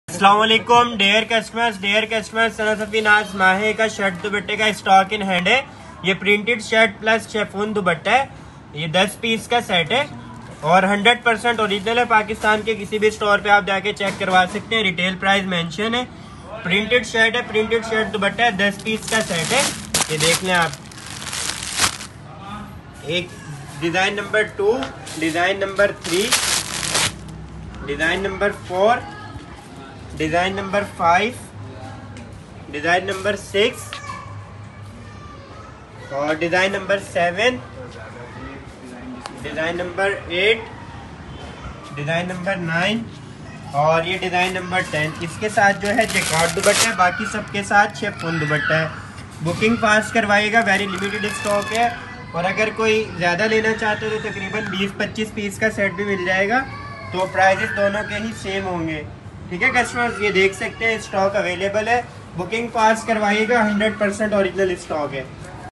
*SANA SAFINAZ Limited Edition ORIGINAL💯 2pc With Chiffon Duppatta Dream my fashion
Collection Alert📣📢🔊huge discount

*SANA SAFINAZ Limited Edition ORIGINAL💯 2pc With Chiffon Duppatta*

Digital printed Chiffon Duppatta

10 Designs,
10 piece Set📦

*Wholesale Rate: 1,400/-*

Booking fast very limited stock

Note:

AGR kisi ki 15 pcs ka set chyie tu mill jye lekin price 1400 he ha

*STOCK AVAILABLE‼️*

Stock Bilkul fresh ha with Orignal inly card and packing
Dream my fashion 💕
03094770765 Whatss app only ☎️